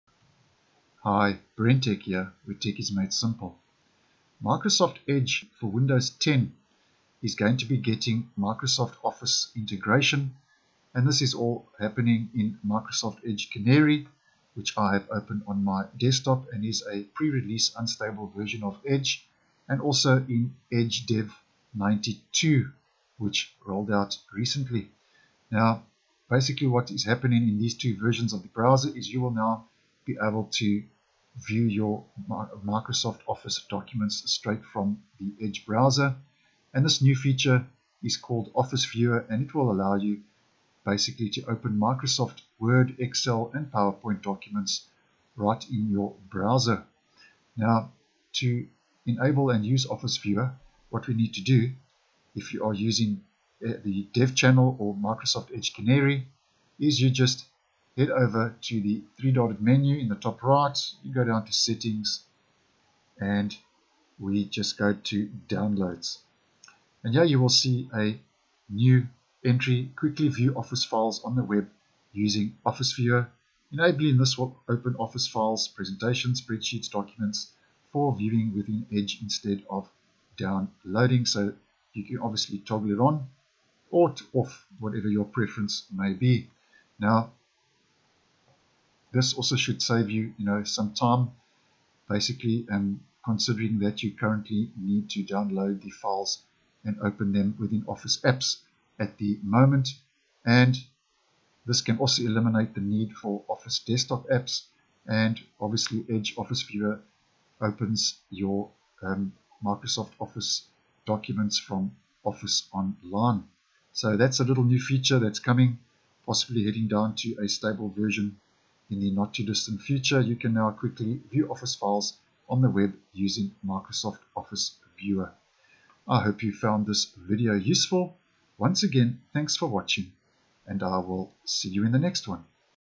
Microsoft Edge is getting Microsoft Office integration
I hope you enjoy this video. Office Viewer will allow you to open Microsoft Word, Excel, and PowerPoint documents right in your browser.